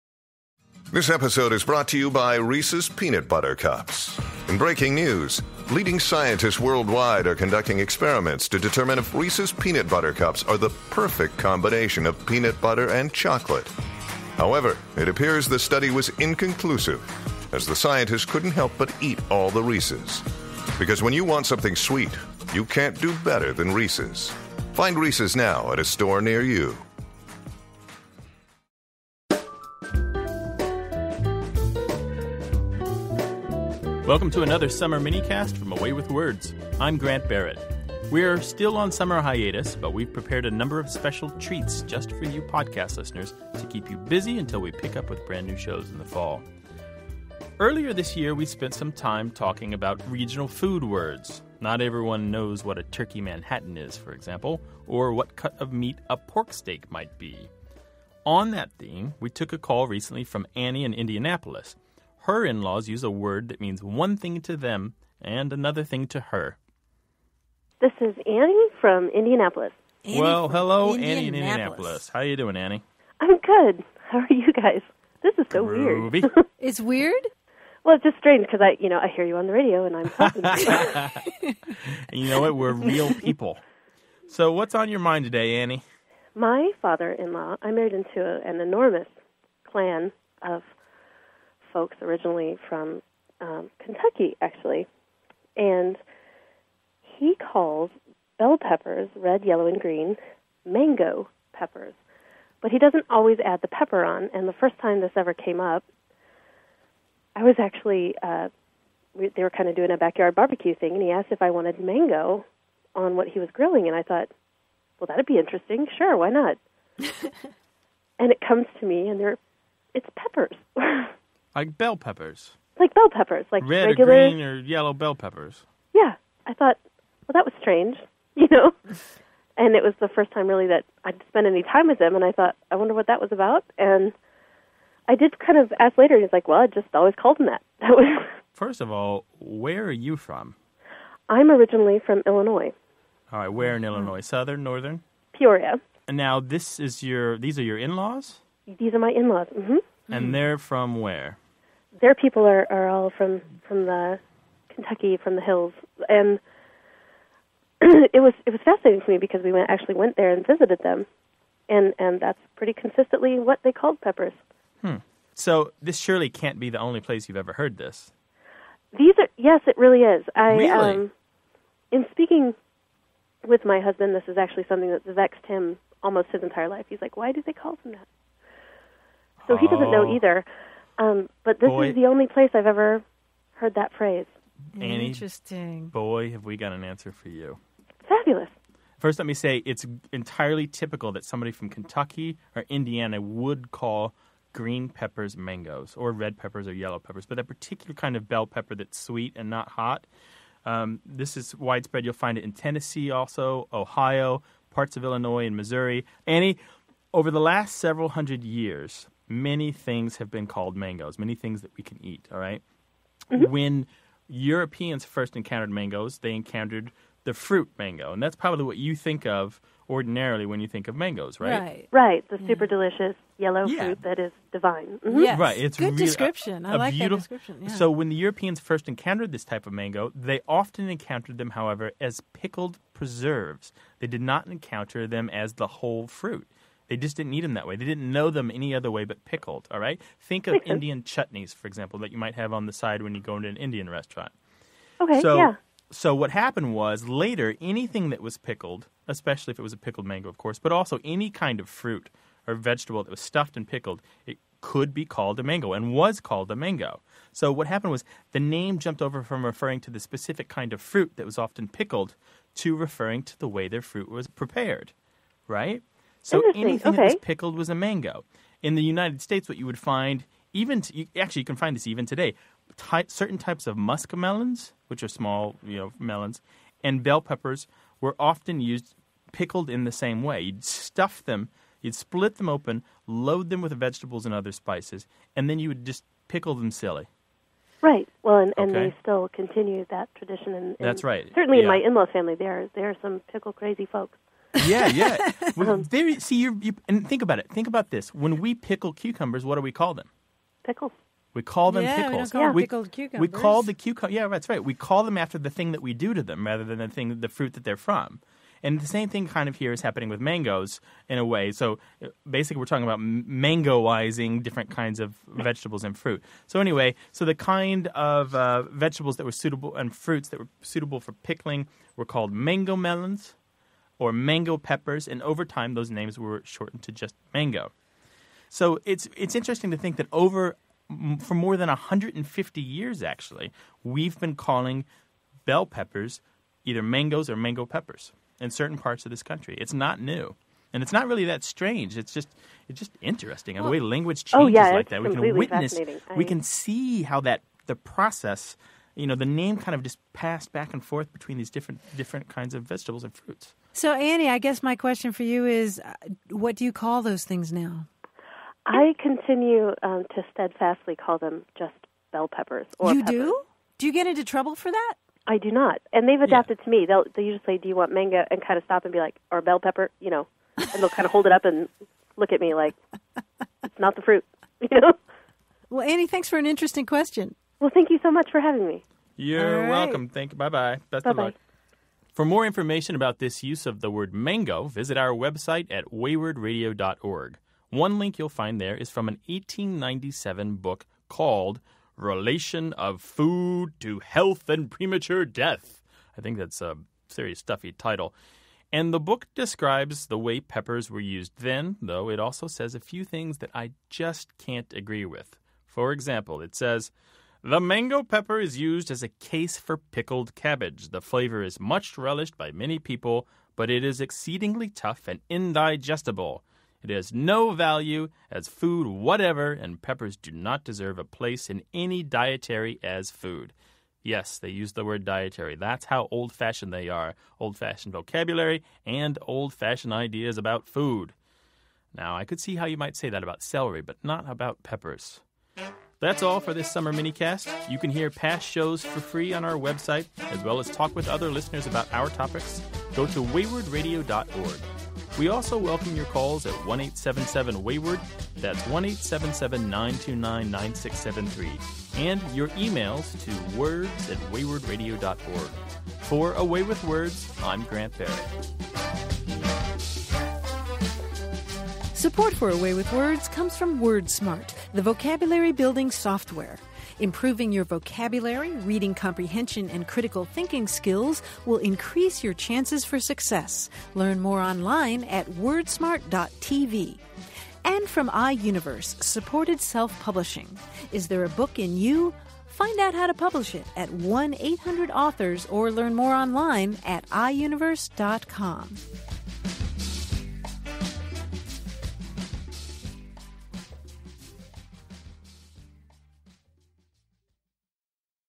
When is a Bell Pepper a Mango? Minicast - 17 Aug. 2008
When is a mango not a mango? Why, when it's a bell pepper, of course! An Indiana listener says she and her Kentucky in-laws have entirely different names for this vegetable. She wants to know why, so we help her sort it out.